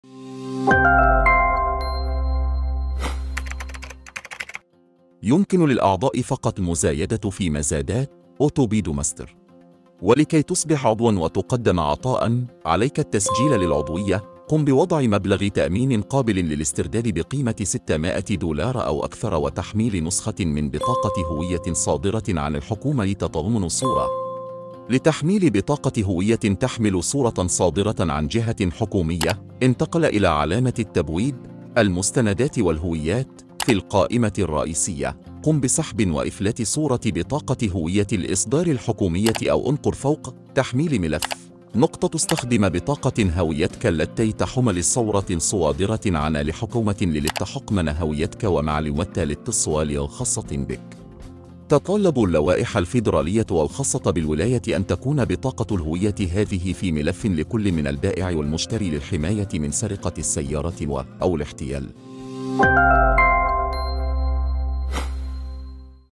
كيف أقوم بتحميل بطاقة الهوية الخاصة بي؟
يمكن للأعضاء فقط المزايدة في مزادات (أوتوبيد ماستر). ولكي تصبح عضوًا وتقدم عطاءً، عليك التسجيل للعضوية، قم بوضع مبلغ تأمين قابل للاسترداد بقيمة 600 دولار أو أكثر وتحميل نسخة من بطاقة هوية صادرة عن الحكومة تتضمن صورة. لتحميل بطاقة هوية تحمل صورة صادرة عن جهة حكومية، انتقل إلى علامة التبويب "المستندات والهويات" في القائمة الرئيسية ، قم بسحب وإفلات صورة بطاقة هوية الإصدار الحكومية أو انقر فوق "تحميل ملف" .تُستخدم بطاقة هويتك التي تحمل صورة صادرة عن الحكومة للتحقق من هويتك ومعلومات الاتصال الخاصة بك. تتطلب اللوائح الفيدرالية والخاصة بالولاية أن تكون بطاقة الهوية هذه في ملف لكل من البائع والمشتري للحماية من سرقة السيارة و/أو الاحتيال.